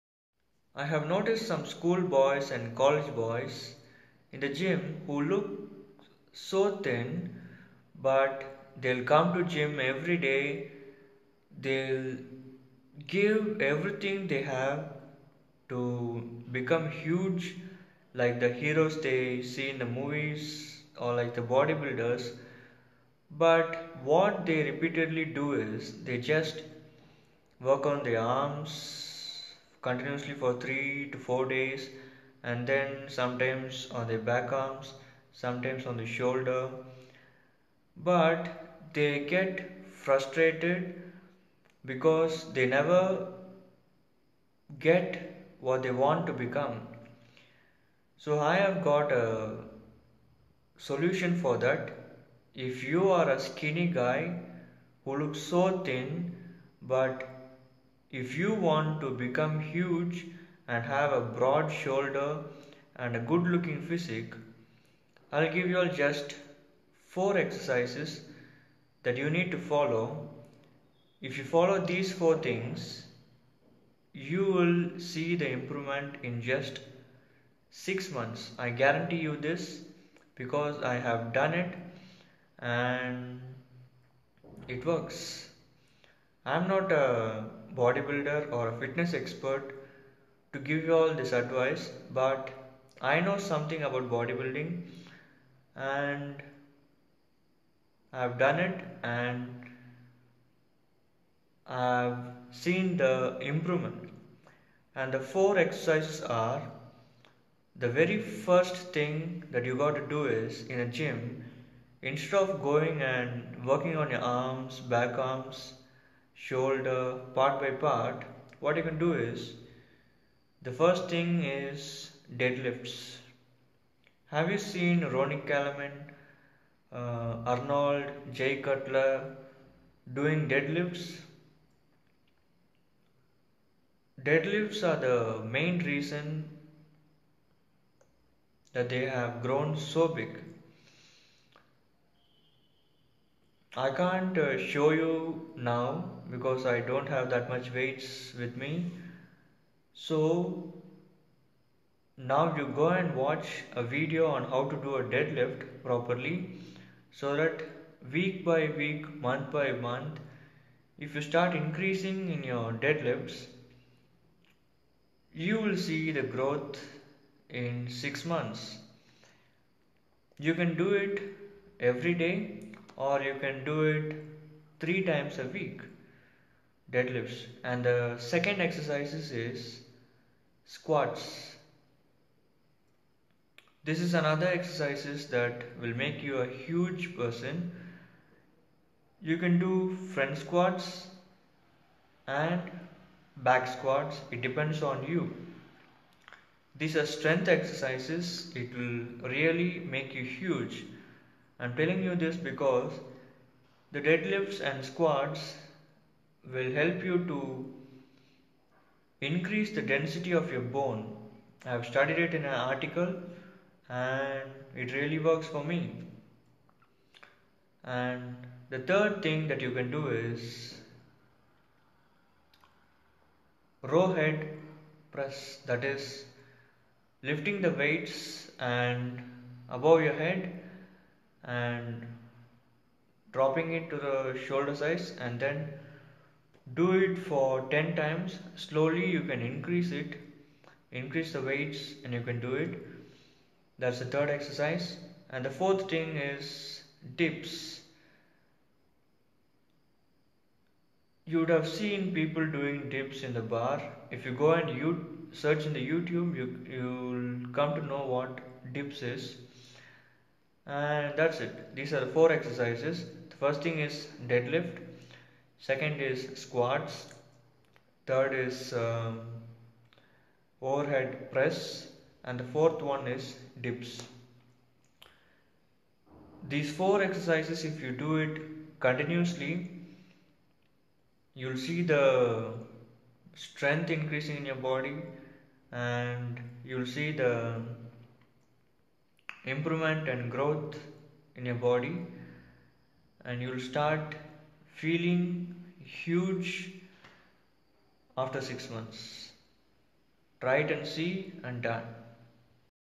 Just four exercises to get broad shoulders and good physique for thin guys.
Just four exercises to get broad shoulders and good physique for thin guys. Just by working on your arms, back arms, shoulders and thighs everyday you can't become huge and to know that four exercises to become huge watch it.